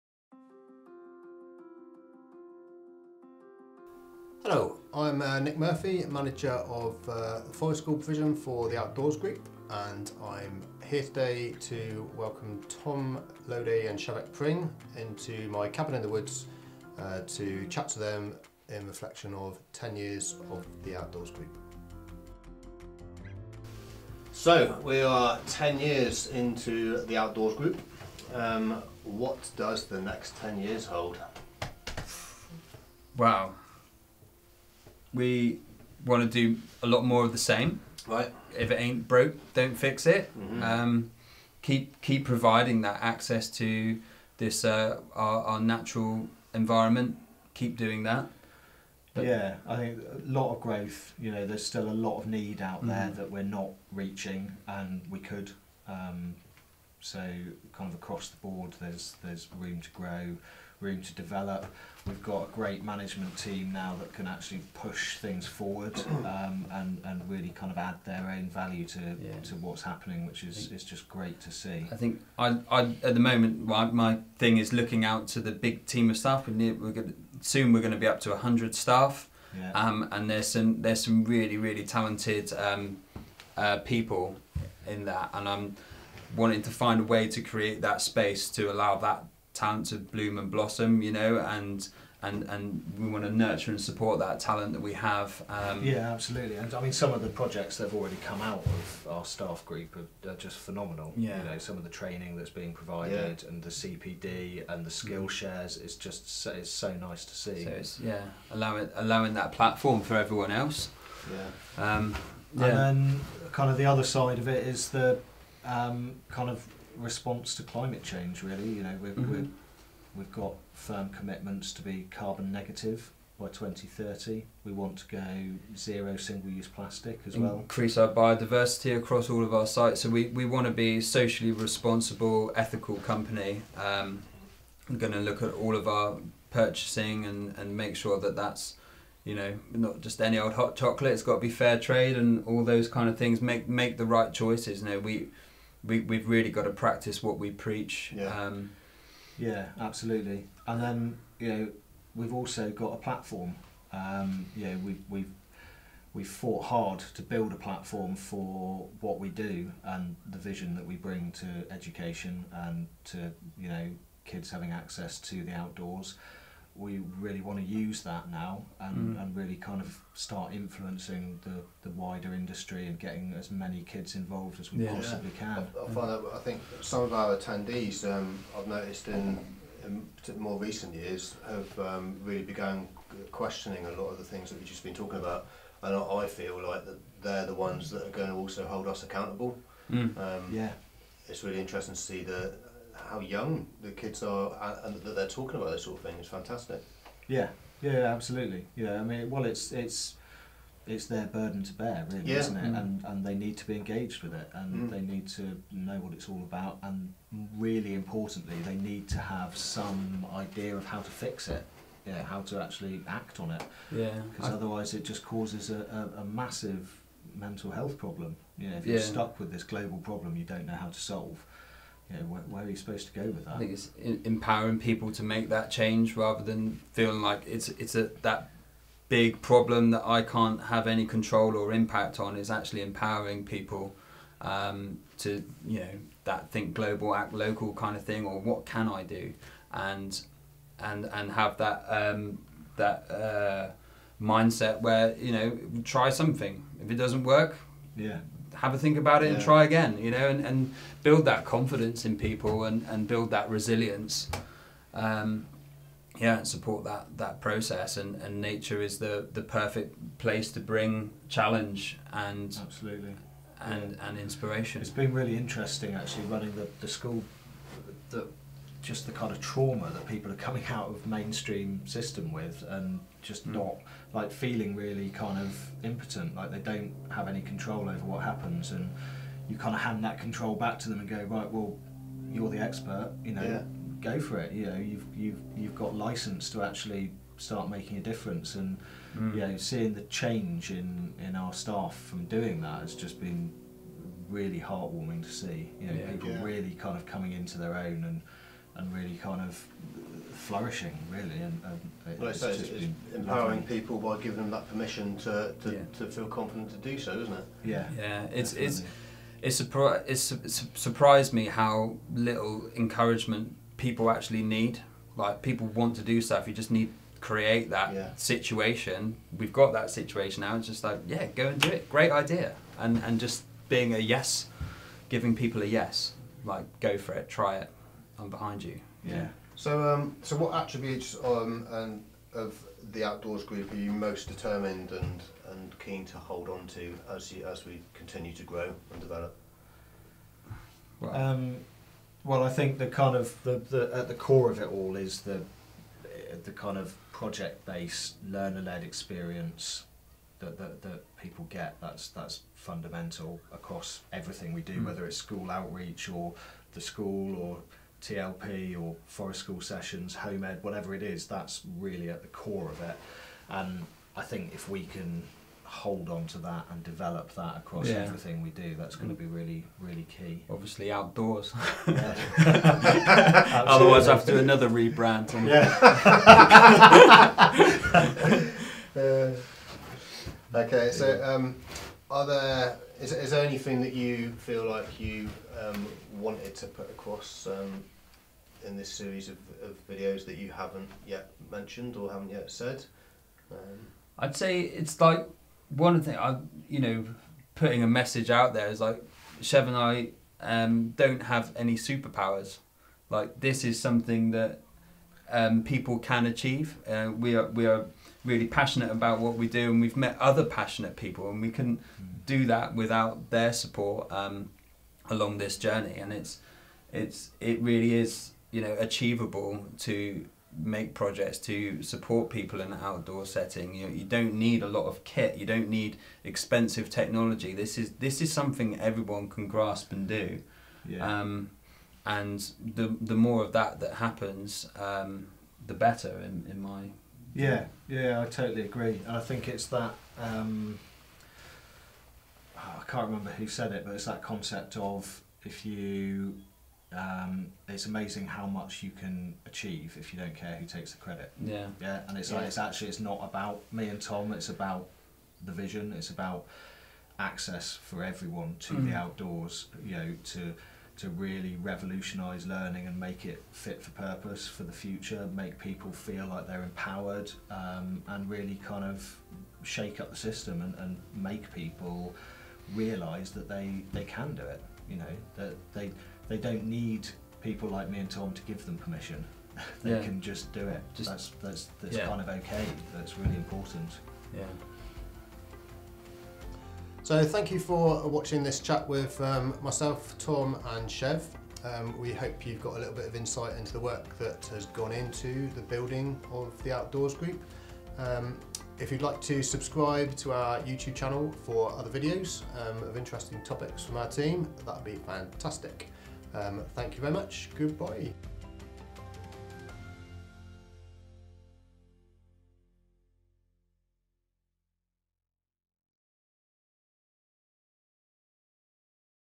Ten Years of TOG - What the Future Holds
This year marks ten years since what is now The Outdoors Group began. We are celebrating this anniversary with a series of short films documenting the journey from running a handful of Forest School sessions in a small woodland just outside Exeter (Devon) to becoming a large and ever growing outdoor education company with sites and provision all around Devon and a reputation for being specialists in working outdoors with children with Autism Spectrum Conditions and social, emotional and behavioural needs.

So please settle in and join our Forest School Manager Nick Murphy as he interviews directors Shevek Pring and Tom Lowday about what the future holds for The Outdoors Group as they enter their second decade of discovering, exploring, learning and playing in the great outdoors.

To find out more about what The Outdoors Group does, head over to our